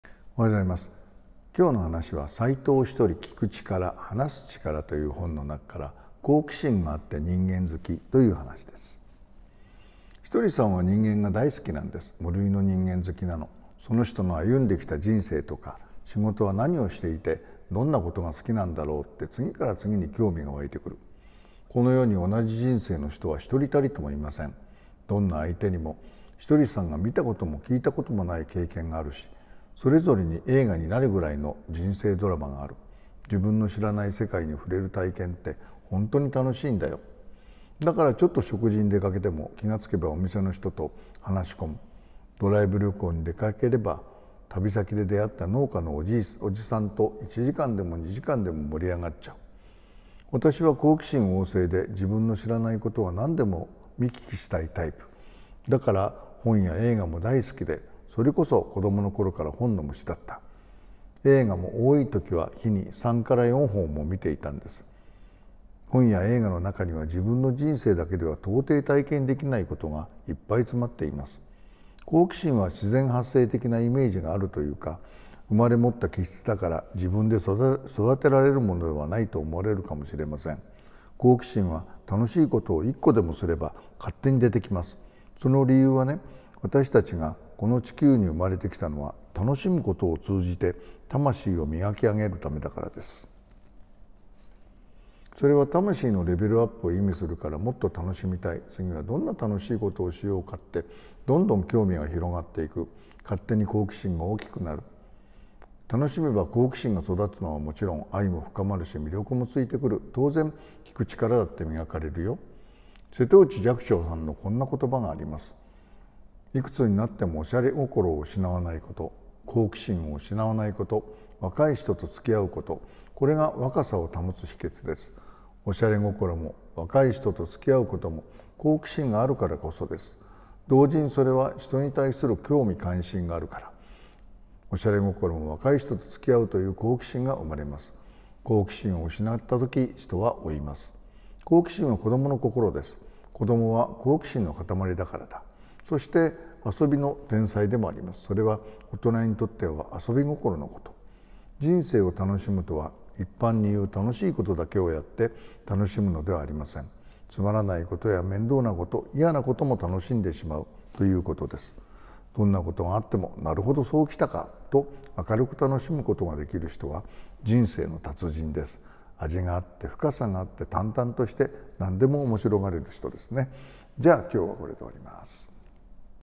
【好奇心があって人間好き】（斎藤一人）
1.一人さんは、人間が大好きなんです。 無類の人間好きなの。 その人の歩んできた人生とか、仕事はなにをしていて、どんなことが好きなんだろうって、次から次に興味が湧いてくる。 この世に、同じ人生の人は1人たりともいません。 どんな相手にも、一人さんが見たことも聞いたこともない経験があるし、それぞれに、映画になるぐらいの人生ドラマがある。 自分の知らない世界に触れる体験って、本当に楽しいんだよ。
2.だから、ちょっと食事に出かけても、気がつけばお店の人と話し込む（笑）。 ドライブ旅行に出かければ、旅先で出会った農家のおじさんと1時間でも2時間でも盛り上がっちゃう（笑）。 私は好奇心旺盛で、自分の知らないことはなんでも見聞きしたいタイプ。 だから本や映画も大好きで、それこそ子どもの頃から本の虫だった。 映画も、多いときは日に3～4本も観ていたんです。 本や映画のなかには、自分の人生だけではとうてい体験できないことが、いっぱい詰まっています。
3.好奇心は、自然発生的なイメージがあるというか、生まれ持った気質だから、自分で育てられるものではないと思われるかもしれません。 好奇心は、楽しいことを1個でもすれば勝手に出てきます。 その理由はね、私たちがこの地球に生まれてきたのは、楽しむことを通じて魂を磨き上げるためだからです。
4.それは魂のレベルアップを意味するから、もっと楽しみたい、次はどんな楽しいことをしようかって、どんどん興味が広がっていく。 勝手に好奇心が大きくなる。 楽しめば好奇心が育つのはもちろん、愛も深まるし、魅力もついてくる。 当然、聞く力だって磨かれるよ。
◆瀬戸内寂聴氏のこんな言葉がある。 『いくつになってもおしゃれ心を失わないこと、好奇心を失わないこと、若い人と付き合うこと。 これが、若さを保つ秘訣です。』 おしゃれ心も、若い人と付き合うことも、好奇心があるからこそだ。 同時にそれは、人に対する興味関心があるから、おしゃれ心も、若い人と付き合うという好奇心が生まれる。 好奇心を失った時、人は老いる。
◆好奇心は子どもの心だ。 子どもは、好奇心のかたまりだからだ。 そして、遊びの天才でもある。 それは、大人にとっては「遊び心」のこと。 人生を楽しむとは、一般にいう楽しいことだけをやって楽しむのではない。 つまらないことや、めんどうなこと、嫌なことも楽しんでしまう、ということ。
◆どんなことも「なるほど、そうきたか」と、明るく楽しむことができる人は人生の達人だ。 味があって、深さがあって、淡々としていて、何でもおもしろがれる人。 別れてすぐに、また会いたいと思わせるような、余韻のある人だ。 いくつになっても… 好奇心があって人間好きな人でありたい。